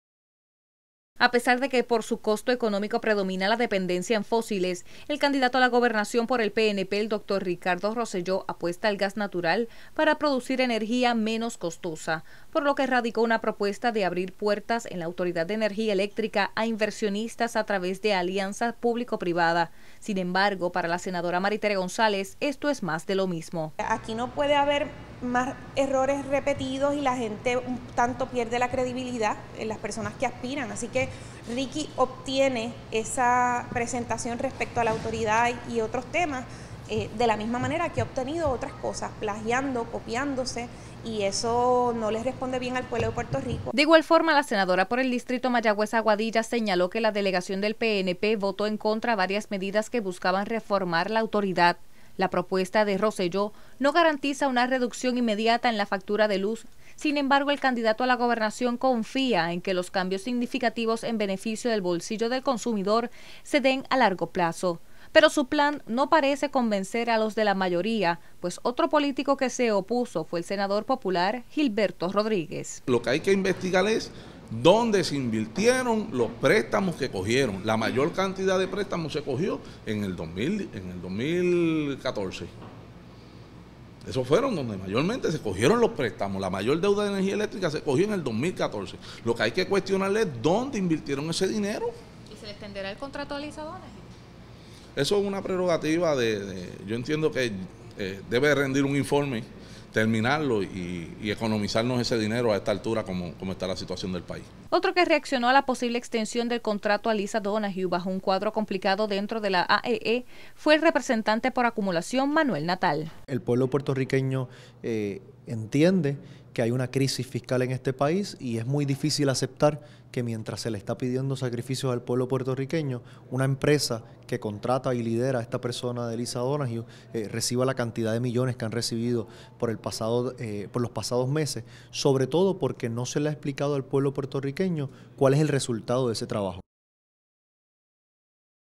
Catalogan como plagio propuestas de Ricardo Rosselló
A solo tres meses para las elecciones generales, siguen lloviendo las propuestas por parte de los candidatos a la gobernación  para remediar  la crisis fiscal lo que provoca opiniones encontradas dentro de los partidos políticos. En esta ocasión el  doctor Ricardo Rosselló apuesta en su propuesta a que inversionistas desarrollen proyectos a largo plazo en la Autoridad de Energía Eléctrica a través de alianzas público privada. A lo que senadores populares catalogaron como plagio.